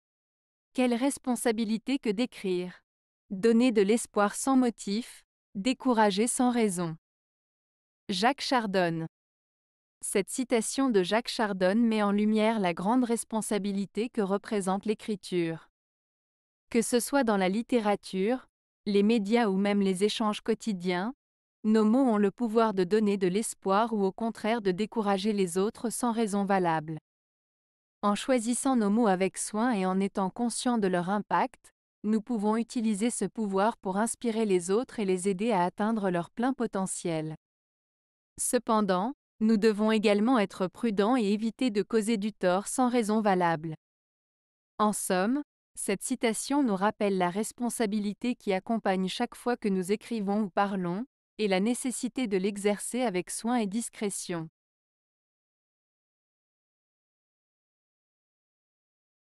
Quelle responsabilité que d'écrire ! Donner de l'espoir sans motif, décourager sans raison.
Cette citation de Jacques Chardonne met en lumière la grande responsabilité que représente l'écriture. Que ce soit dans la littérature, les médias ou même les échanges quotidiens, nos mots ont le pouvoir de donner de l'espoir ou au contraire de décourager les autres sans raison valable. En choisissant nos mots avec soin et en étant conscients de leur impact, nous pouvons utiliser ce pouvoir pour inspirer les autres et les aider à atteindre leur plein potentiel. Cependant, nous devons également être prudents et éviter de causer du tort sans raison valable. En somme, cette citation nous rappelle la responsabilité qui accompagne chaque fois que nous écrivons ou parlons, et la nécessité de l'exercer avec soin et discrétion.